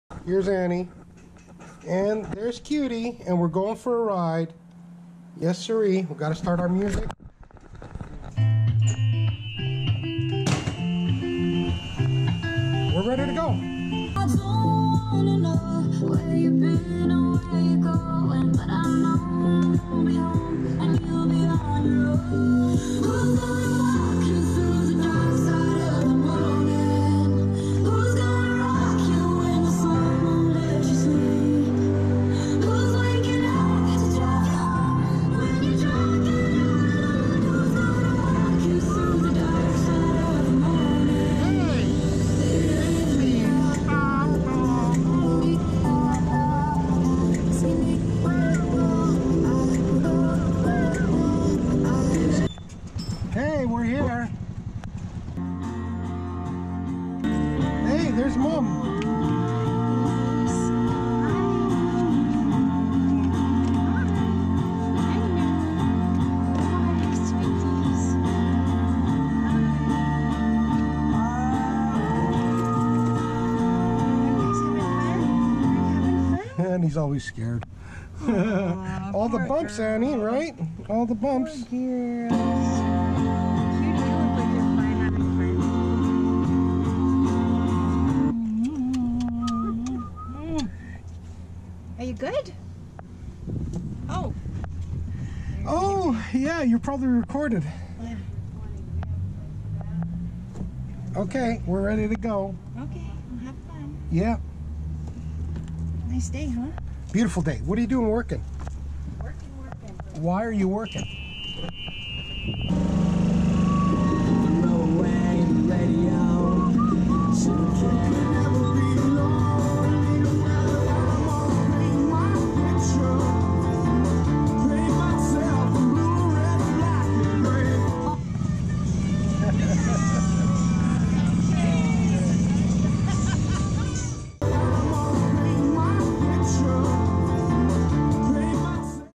Golf Cart allowed on the Galveston Island Seawall (with dogs)
I took the dogs to see Joanne while she was volunteering. They all loved the Cutie factor.  See you in 2020 Galveston.